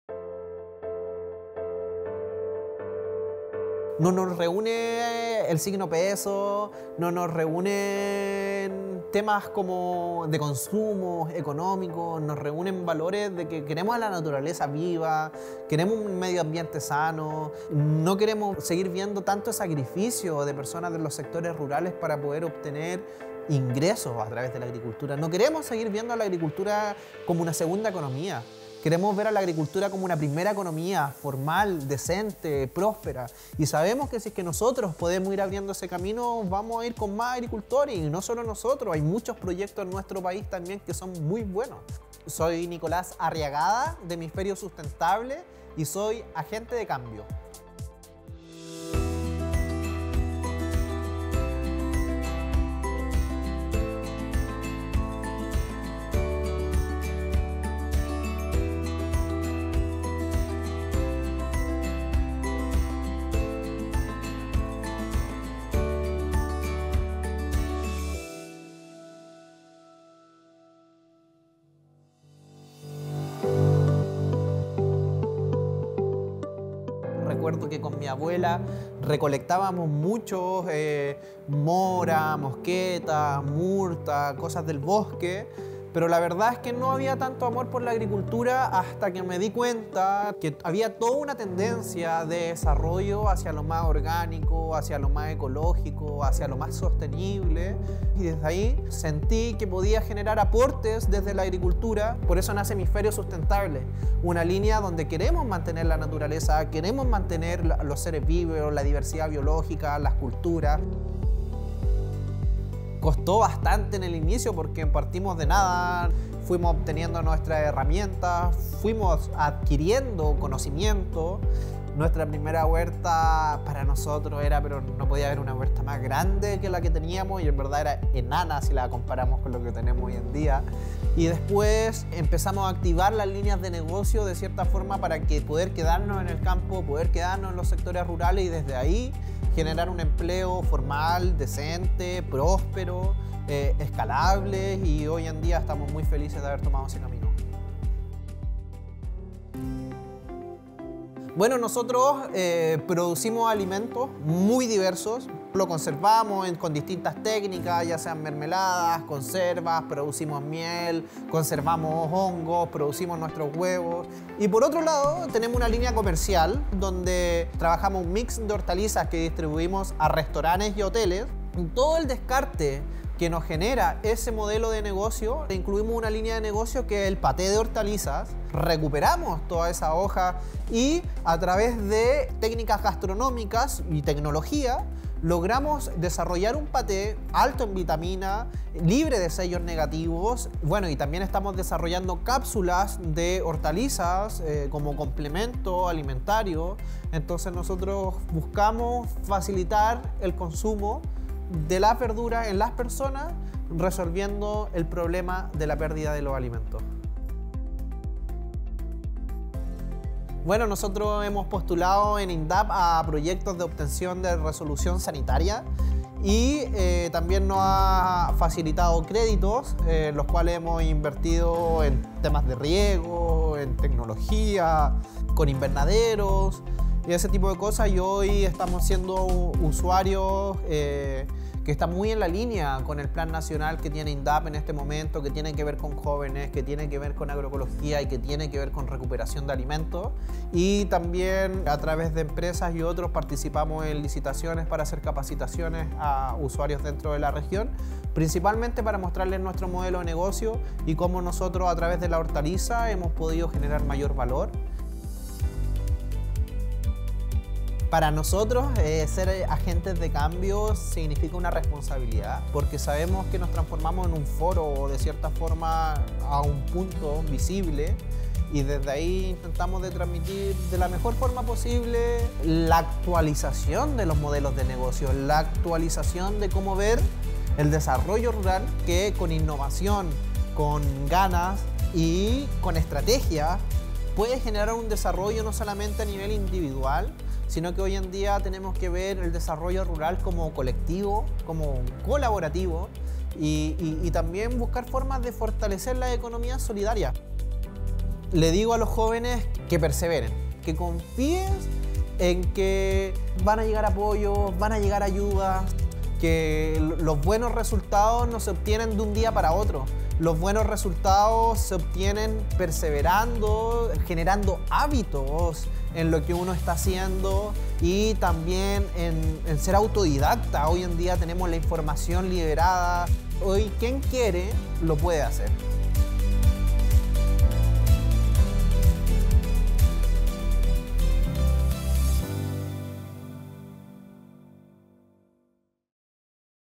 Agente de Cambio - Capítulo 6, Nicolas Arriagada
Nicolas Arriagada de la comuna de Pucón, en la Región de La Araucanía, es el protagonista del 6️⃣ capítulo de AgenteDeCambio🌱.

A través de @hemisferiosustentable, este joven agricultor se dedica a la producción de hortalizas de hoja, las cuales luego las comercializa en el Mercado Agroecológico de la misma comuna y también vende a restoranes y locales de alimentación de su sector. Su producción es 100% libre de químicos y orgánica.

Estamos por una agricultura sostenible y resiliente 🌱